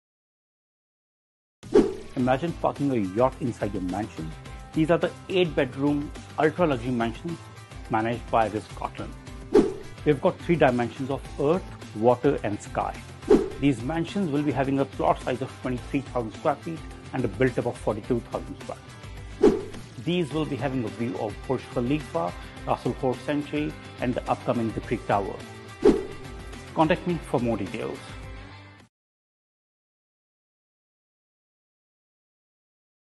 Ritz Carlton Mansions
Imagine parking a yacht inside a Mansion! Cool right🤩🤩

Ritz Carlton has designed 14 ultra luxury and premium Mansions with 8 bedrooms, not only this but with  the latest 3 dimensions technology of earth water and sky around the space to magnify the luxury experience of your living and also the magnificent views of the highly hyped The Burj Khalifa, Rasul Khor Sanctuary and the upcoming The Creek Towers.

A total plot size of 23,000 sq. ft and a built up of 42,000 sq. ft will surely enhance the lifestyle of your dreams. 😍